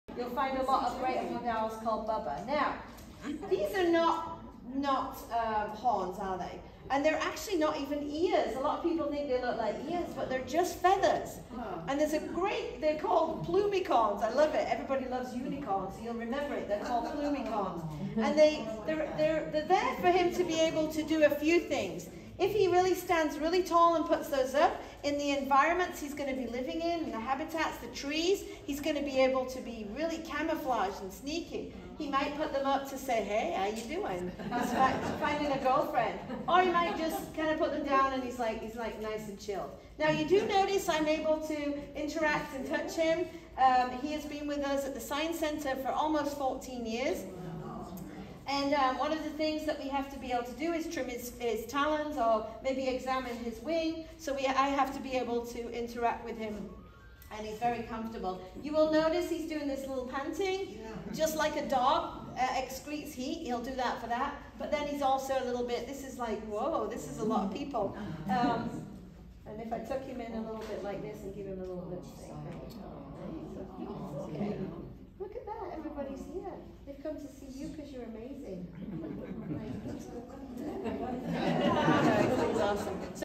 Bubba the Great Horned Owl
Presentation by Shell Webster, Education Coordinator, Marine Science Center, Ponce Inlet. The crowd really enjoyed seeing Bubba the Great Horned Owl and learning all about him. He only weighs about 3 1/2 pounds, was rehabbed but couldn’t be released back to the wild because he flunked flight school, so he became an ambassador. He been there 14 years.
Seen at the Lyonia Center's Scrub-jay and Wildlife Festival on February 12, 2022 in Deltona Florida.
The Friends are a project of the West Volusia Audubon Society